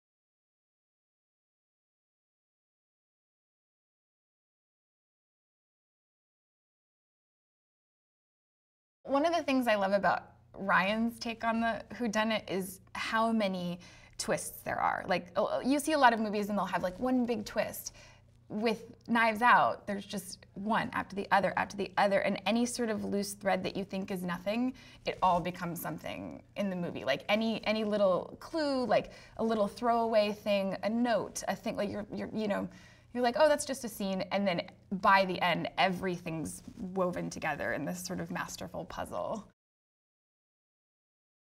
Knives Out - Itw Riki Lindhome (official video)
Knives Out is a 2019 American black comedy mystery film written, produced and directed by Rian Johnson. Described as a modern take on the murder mystery genre, the film follows a family gathering gone horribly awry, after the family patriarch's death leads a master detective to investigate. The film stars an ensemble cast, including Daniel Craig, Chris Evans, Ana de Armas, Jamie Lee Curtis, Michael Shannon, Don Johnson, Toni Collette, Lakeith Stanfield, Katherine Langford, Jaeden Martell, and Christopher Plummer. The film had its world premiere at the Toronto International Film Festival on September 7, 2019 and is scheduled to be theatrically released in the United States on November 27, 2019, by Lionsgate Films and in France on the same day by Metropolitan FilmExport

Synopsis :
Wealthy crime novelist Harlan Thrombey invites his extended dysfunctional family to his remote mansion on his 85th birthday in hopes of reuniting them all. However, the day after the birthday party, Harlan is found dead by members of the family. The police, along with experienced detective Benoit Blanc, are called in to investigate. Soon enough, everyone in the family becomes a suspect in his death.

Knives Out
Written and Directed by Rian Johnson
Produced by Rian Johnson, Ram Bergman
Starring Daniel Craig, Chris Evans, Ana de Armas, Jamie Lee Curtis, Michael Shannon, Don Johnson, Toni Collette, Lakeith Stanfield, Katherine Langford, Jaeden Martell, Christopher Plummer
Cinematography : Steve Yedlin
Edited by Bob Ducsay
Production company : Media Rights Capital,  T-Street
Distributed by Lionsgate (United States), Metropolitan FilmExport (France)
Release date : September 7, 2019 (TIFF), November 27, 2019 (United States)
Running time : 130 minutes

(Source : Wikipedia)